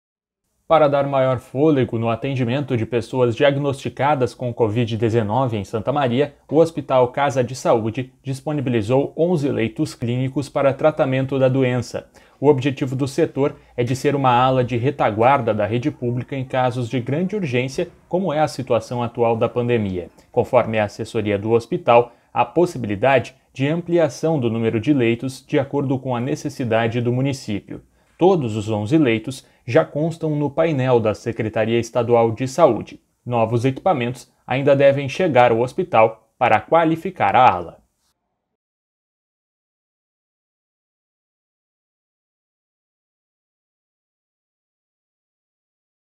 Ala emergencial da Casa de Saúde recebe pacientes com Covid-19
Unidade disponibiliza 11 leitos clínicos, sendo que nove já estão ocupados. Há possibilidade de ampliação do setor